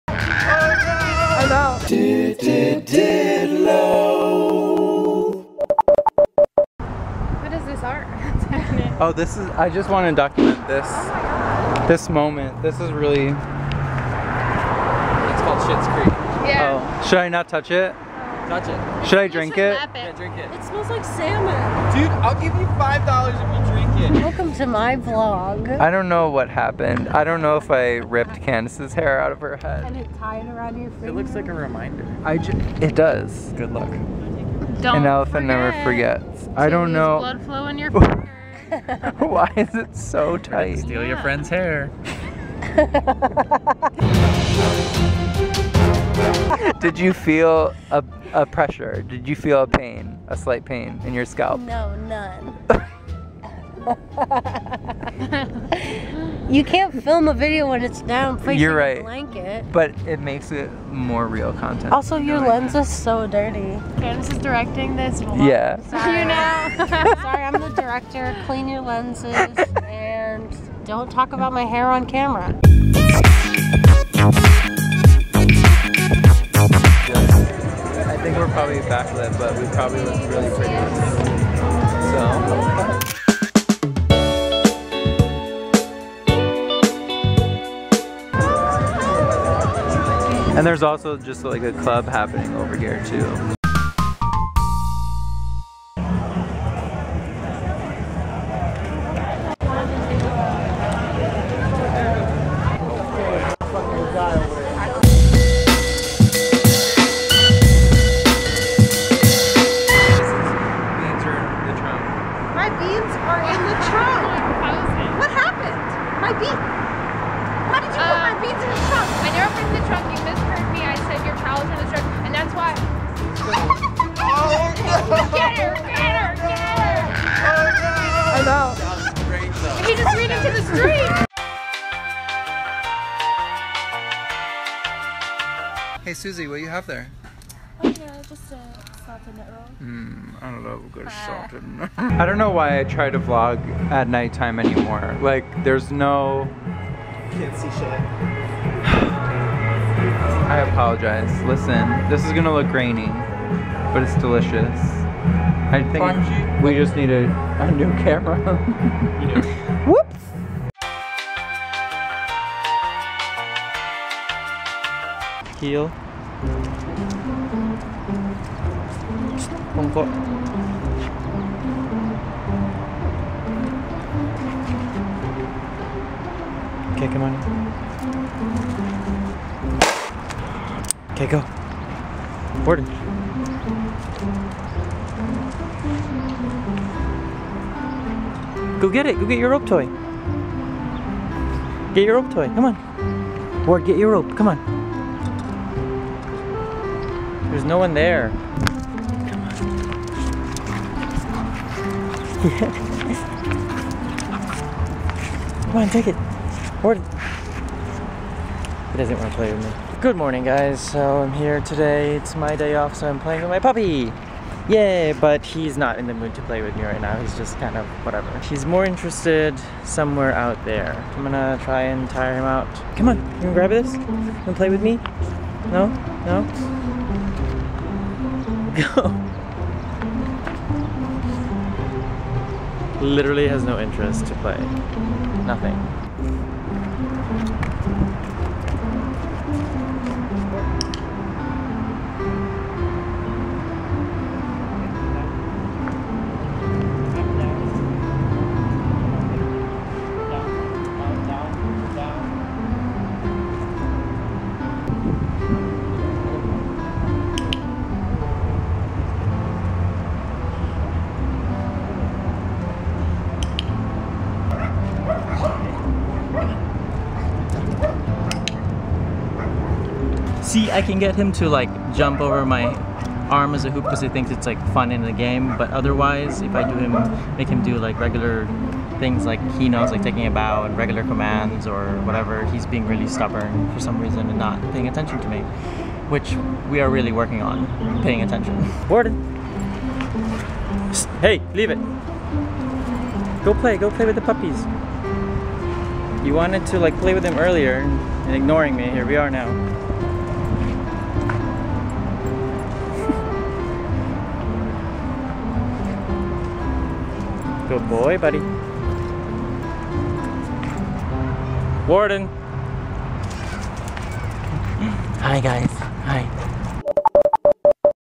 I Literally Snatched Her Hair From Her Scalp
In this DITLO*, I went to the beach with Shannon, Rob, and Candace, found a dirty puddle I wanted to lick, shamed by Candace for having a dirty lens, literally snatched her hair from her scalp, mediated a fight over beans, got a good nut with Suzy, got delicious Greek food you can't see with Miles and Ken, and Warden refused to play.

*DITLO stands for Day In The Life Of (me...and my friends and family)
OTHER CHANNELS AND STUFF
Follow Nic places!
Follow Ken places!